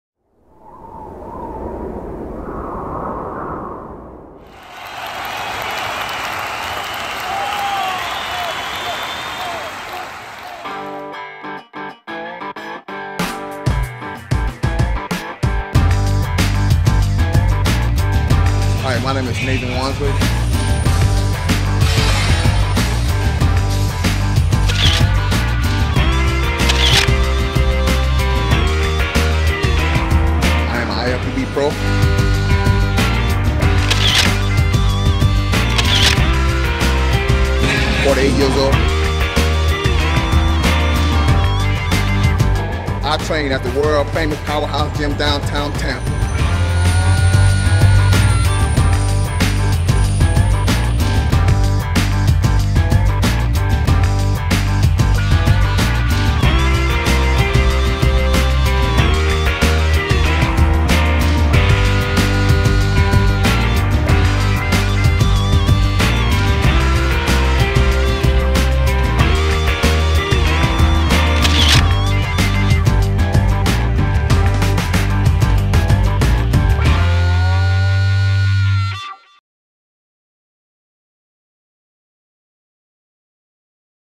Nathan Wonsley at PowerHouse Gym Downtown Tampa
48 year old Nathan Wonsley at the Southern Mecca, PowerHouse Gym Downtown Tampa